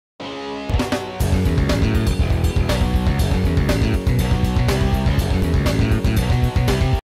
Channel Intro (Version 3)
My third, and final channel intro/trailer. I only re-did it because I decided to delete all my JFK assassination videos. So I made a second channel for just those.